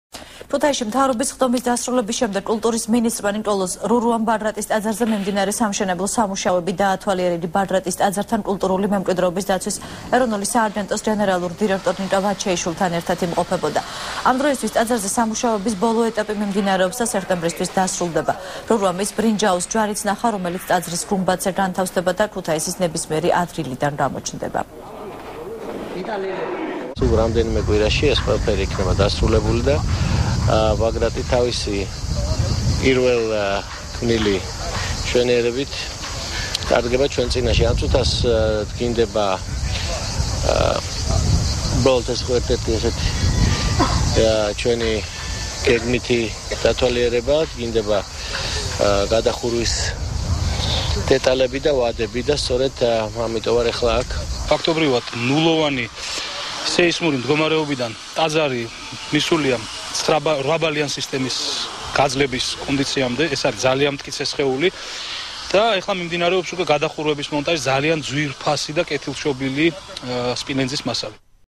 ახალი 6 | რურუა ბაგრატის ტაძარში | 01.08.12
ქუთაისში მთავრობის სხდომის დასრულების შემდეგ კულტურის მინსტრმა ნიკოლოზ რურამ ბაგრატის ტაძარზე მიმდინარე სამშენებლო სამუშაოები დაათვალიერა. იგი ბაგრატის ტაძართან კულტურული მემკვიდრეობის დაცვის ეროვნული სააგენტოს გენერალურ დირექტორ  ნიკა ვაჩეიშვილთან ერთად   იმყოფებოდა.  ამ დროისთვის ტაძარზე სამუშაოების ბოლო ეტაპი მიმდინარეობს და  სექტემბრისთვის დასრულდება. რურუამ ის ბრინჯაოს ჯვარიც ნახა, რომელიც ტაძარის გუმბათზე განთავსდება და ქუთაისის ნებისმიერი ადგილიდან გამოჩნდება.